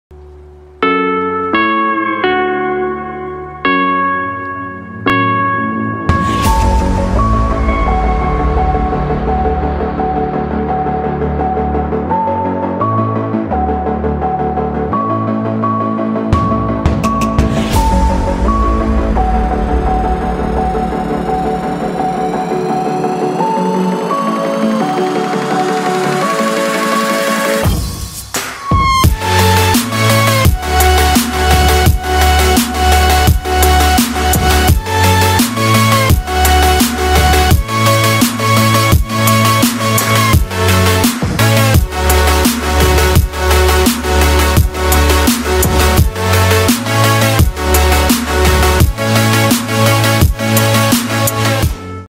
Big graffiti on containers
20 Containers 40ft. big transformed into a gigantic historic graffiti in the city of Drammen by the artists Mikael Noguchi and Eric Ness Christiansen.

Project done for the Port of Drammen.
Edit: Gabrielle Dadgostar
Video: Shujah Malik